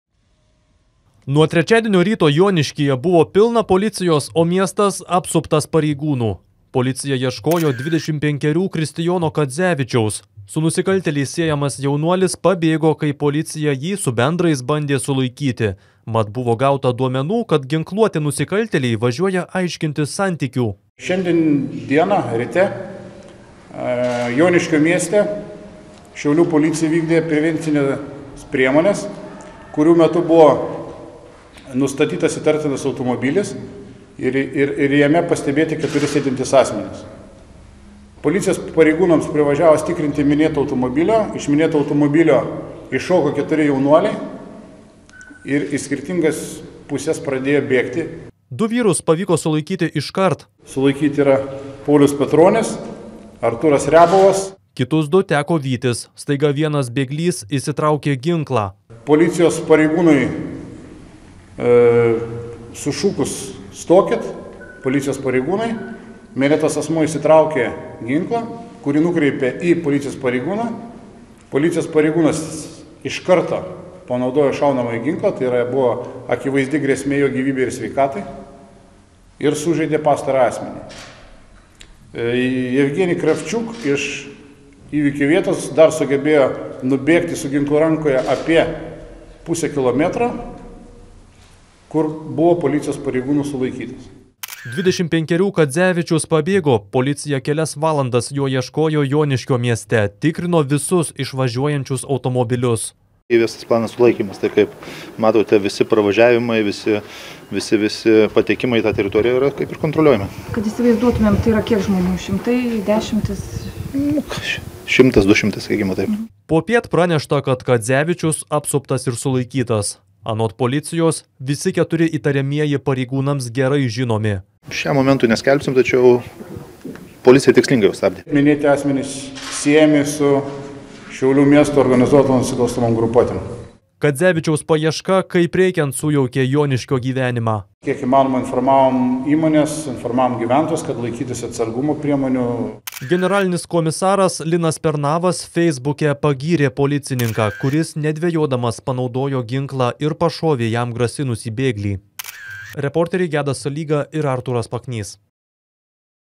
Linas Pernavas pagyrė Joniškyje įtariamąjį pašovusį pareigūną
Pavojingos gaudynės Joniškyje – po susišaudymo su policijos pareigūnais paspruko ginkluotas ir pavojingas, su nusikalstamu pasauliu siejamas vyras. Dar vienas buvo pašautas, jam pabėgti nepavyko. Joniškį užgulė gausios policijos pajėgos – gyventojai perspėti saugotis. Po kelių valandų bėglys sulaikytas.